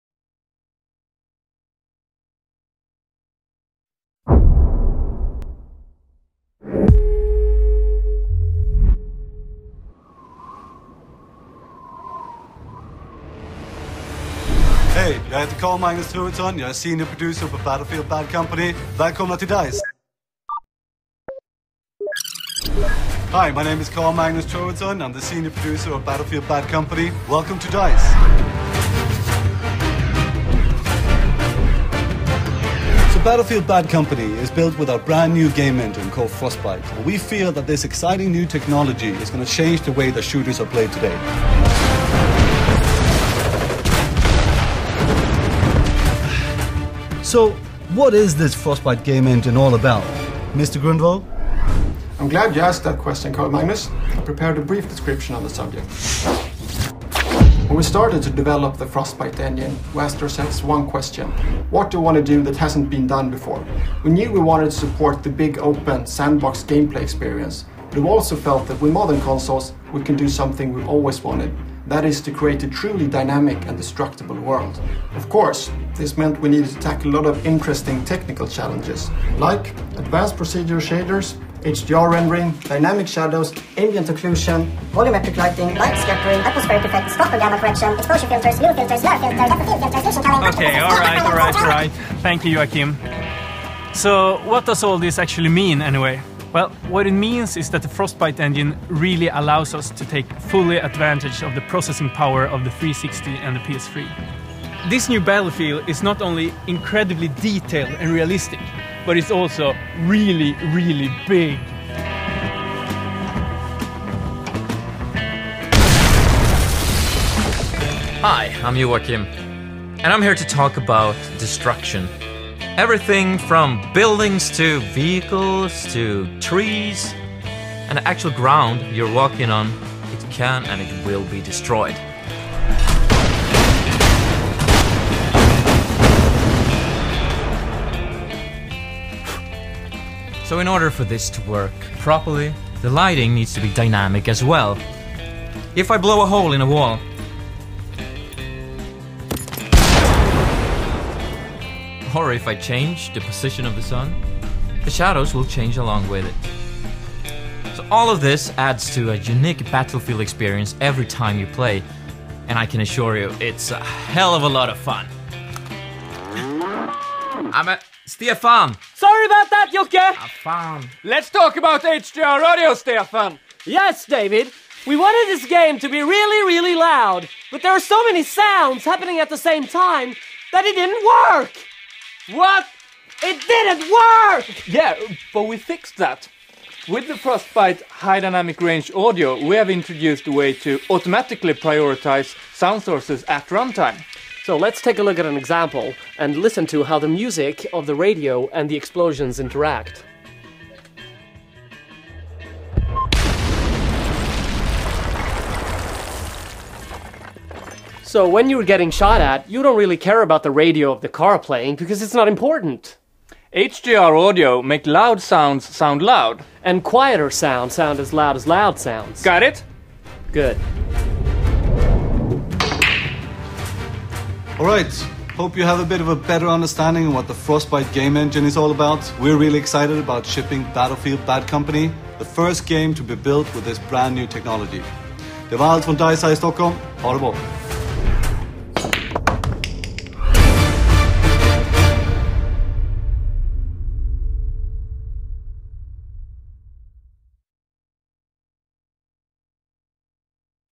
Battlefield: Bad Company - Frostbite Engine
Developers of Battlefield: Bad Company talk about the Frostbite Engine.